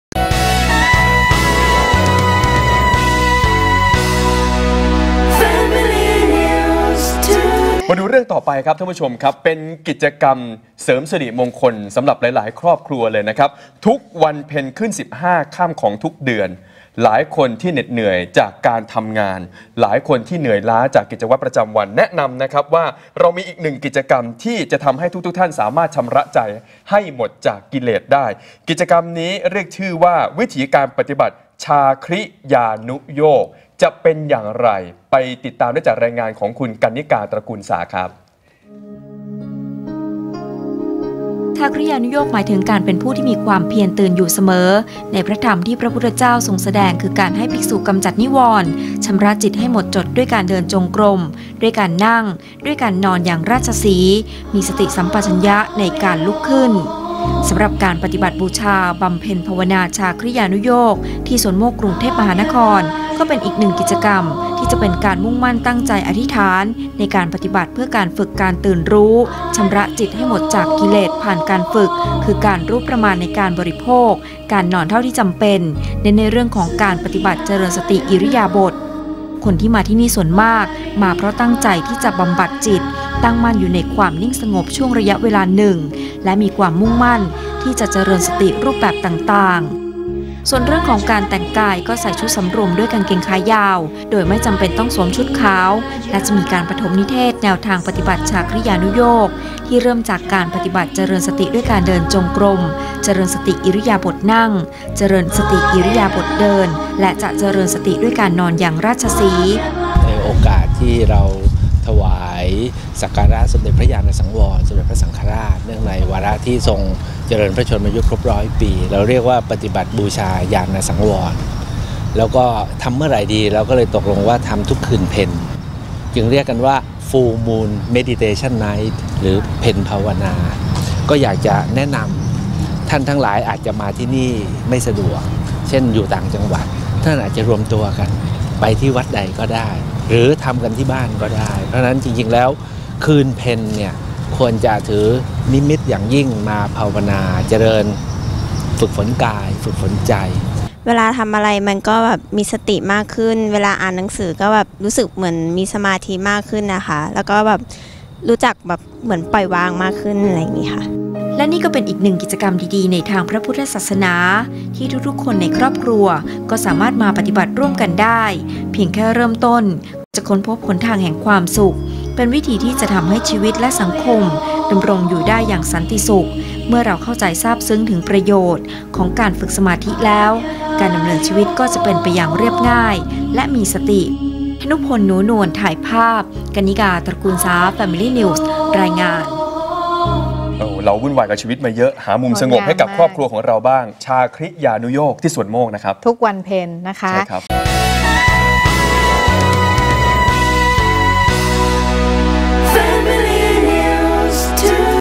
เพ็ญภาวนา ชาคริยานุโยค
กิจกรรมเพ็ญภาวนา ชาคริยานุโยค จัดขึ้นทุกวันเพ็ญขึ้น 15 ค่ำ (วันพระใหญ่) ณ หอจดหมายเหตุพุทธทาส อืนทปัญโญ

วิถีการปฏิบัติ “ชาคริยานุโยค”  เป็นการปรารภความเพียรแห่งธรรมอันเป็นเครื่องตื่น ชำระจิตให้หมดจดจากกิเลส ผ่านการเจริญสติอิริยาบถเดินและนั่งในยามแรกแห่งราตรี ( ๑๘.๐๐ – ๒๒.๐๐ น.), แล้วนอนอย่างราชสีห์ (ตะแคงขวา เท้าเหลื่อมเท้า มีสัมปชัญญะในการลุกขึ้น) ในยามกลาง (๒๒.๐๐ – ๐๒.๐๐ น.) ก่อนจะภาวนาในยามสุดท้าย ( ๐๒.๐๐ – ๐๖.๐๐ น.) ด้วยอิริยาบถเดินและนั่งอีกครั้ง

การปฏิบัติไม่จำกัดวิธีการ หรือ สายปฏิบัติ
สนใจเข้าร่วมกิจกรรม อ่านรายละเอียดได้ที่